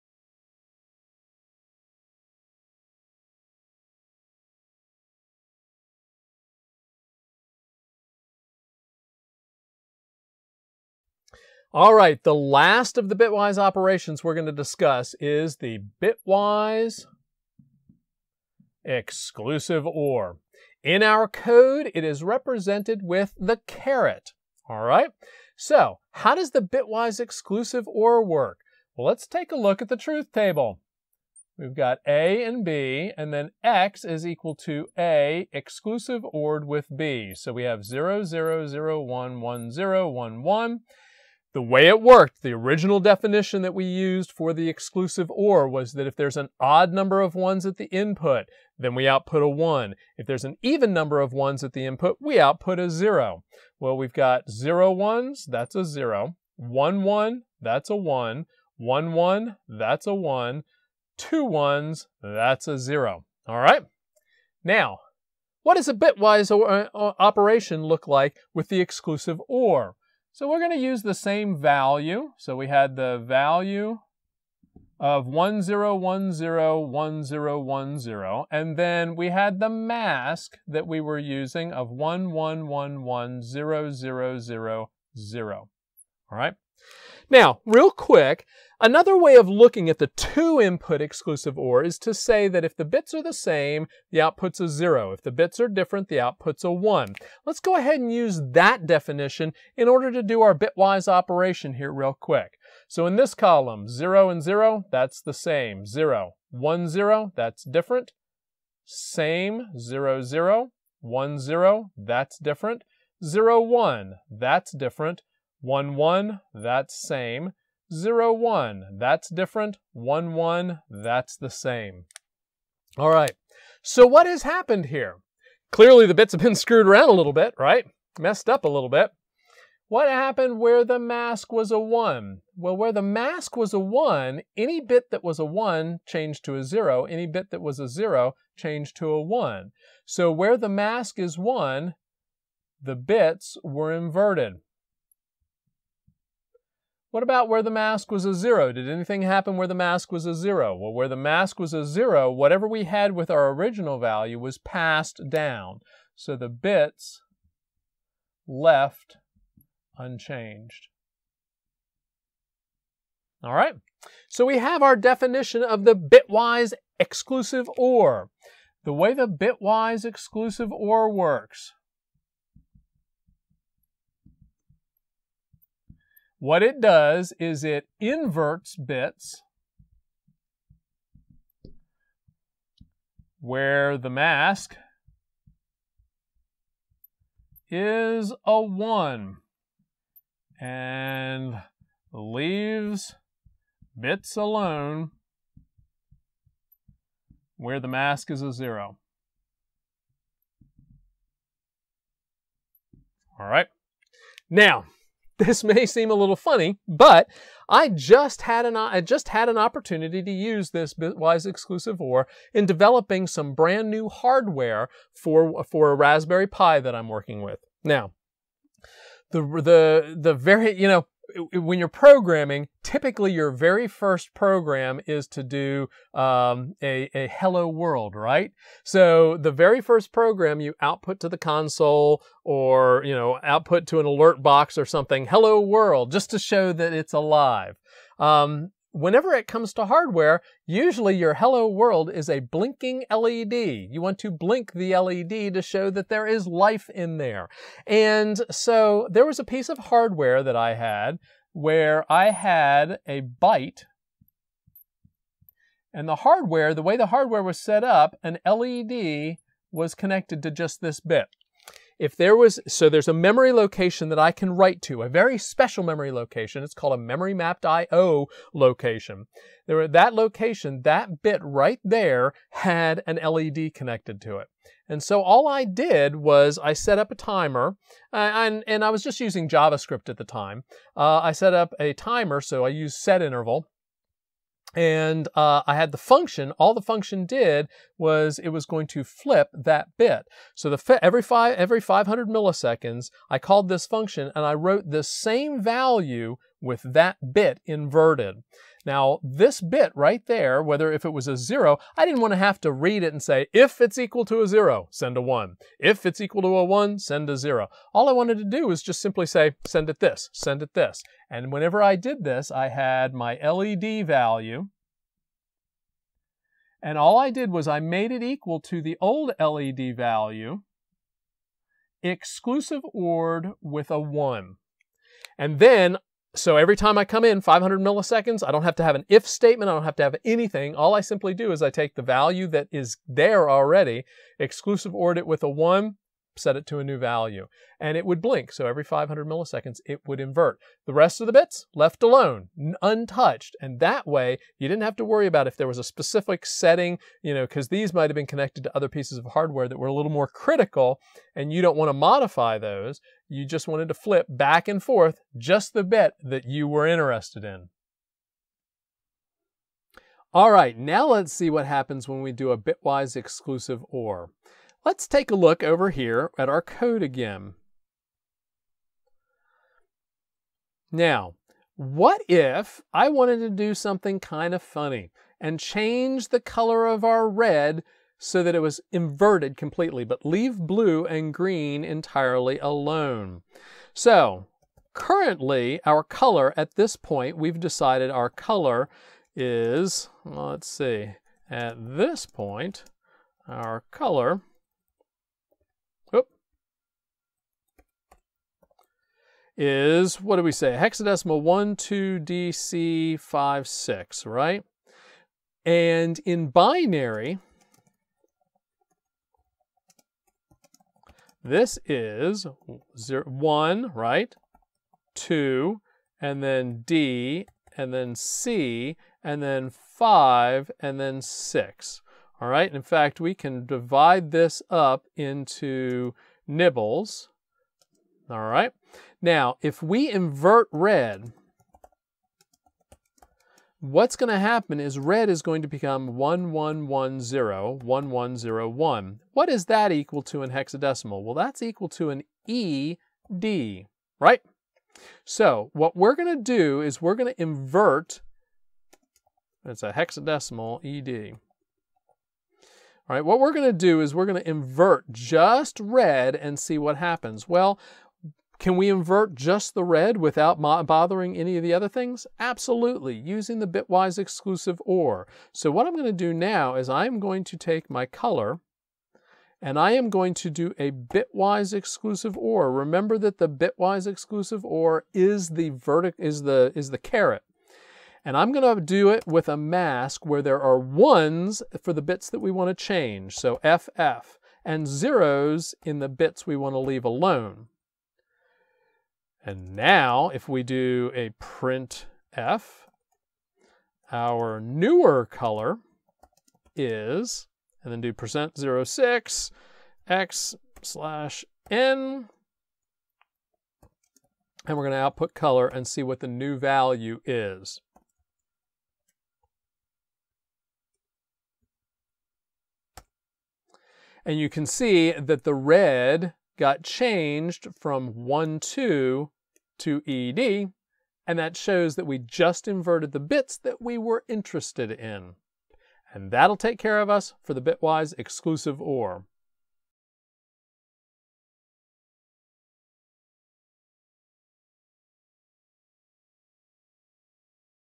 Ep 049: Flipping Bits using the Bitwise-XOR
Inverting or flipping the bits of an integer is the third and last method of bit-bashing we will discuss. The bitwise inverse allowed us to invert all of the bits, but the bitwise-XOR allows us to use a mask to identify which bits to flip and which to leave alone.